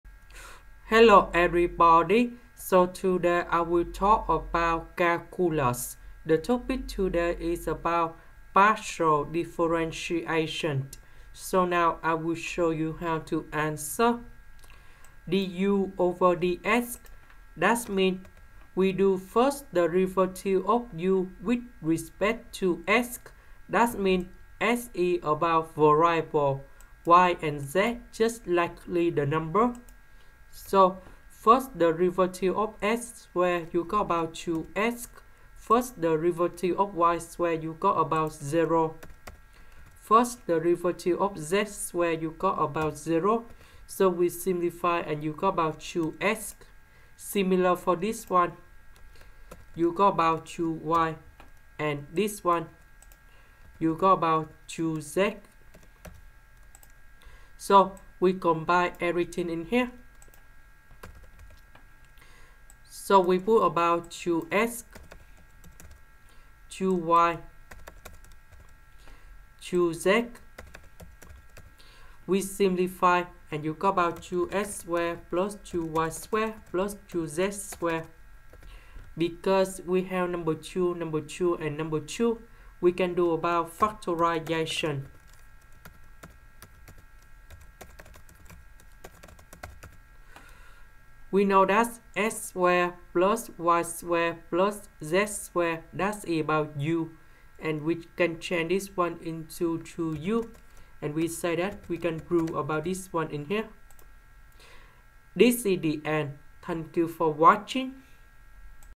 Calculus Help: If u=x^2+y^2+z^2,show that x du/dx+y du/dy+z du/dz=2u - Partial Differentiation
Here is the technique how to prove and solve the partial differentiation